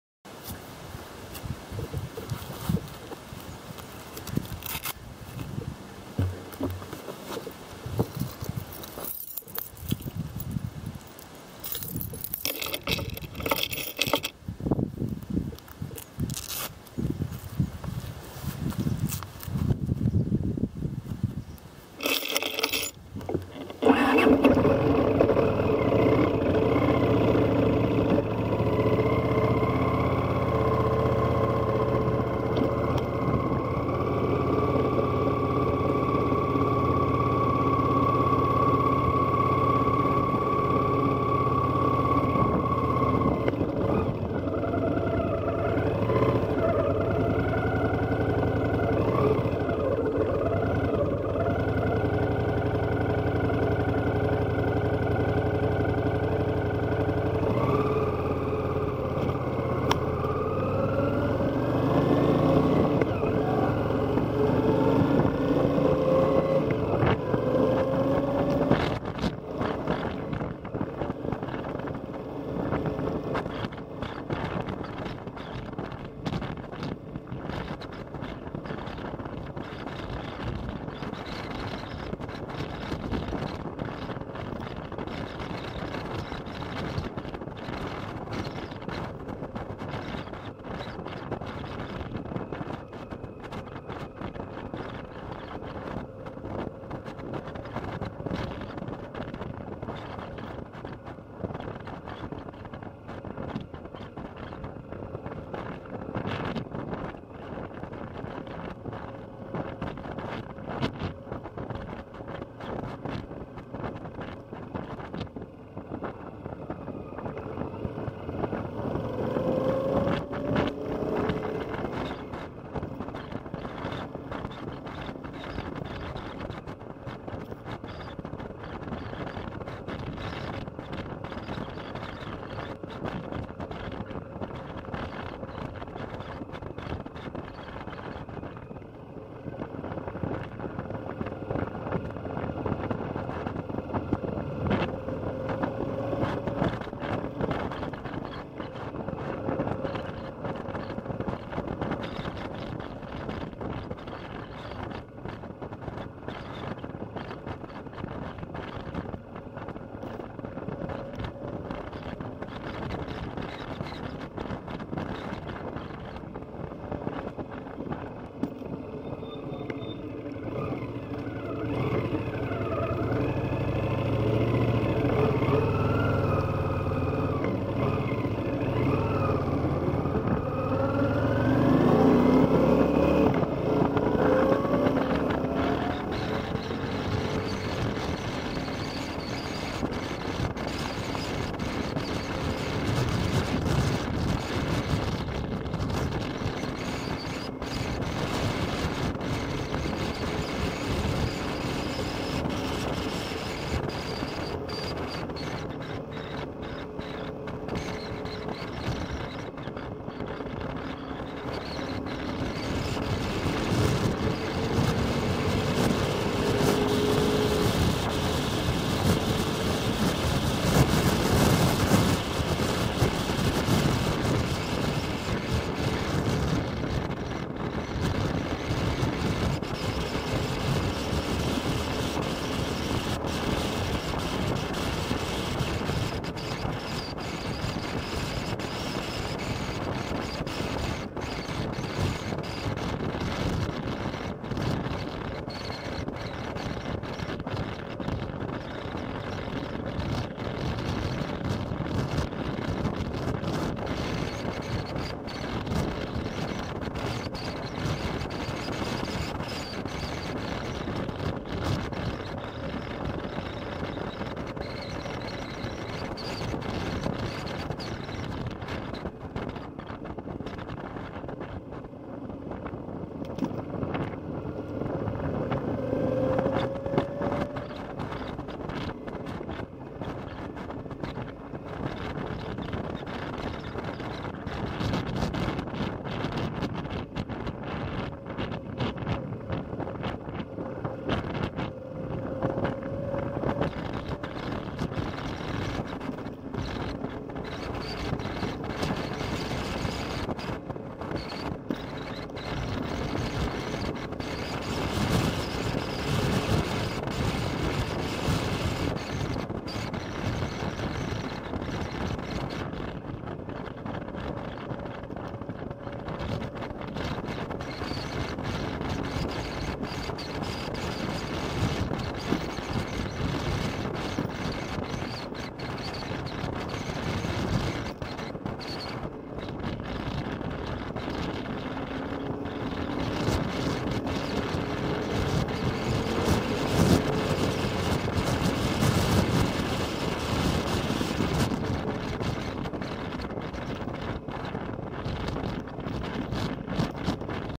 Start of spring run out on CX500
If you've not been to the village, here's a run down.Village shop 1.36; faulty speed sign at 1:46 ;); Swan pub 1:52, Grendon Underwood school at 2:13: Village hall at 2:20; Shakespeare house at 2:36; Village Church at 2:57- onto Edgcott road.Entrance to Springhill, and Springhill prison at 3:58.Into Edgcott at 4:06 Out at the end